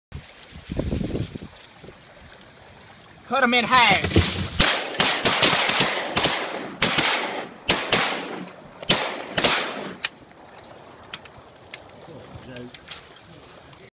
Riverbend duck hunting club 2014 hunt clip
A short clip of hunting footage from a great duck hunt at RBDC.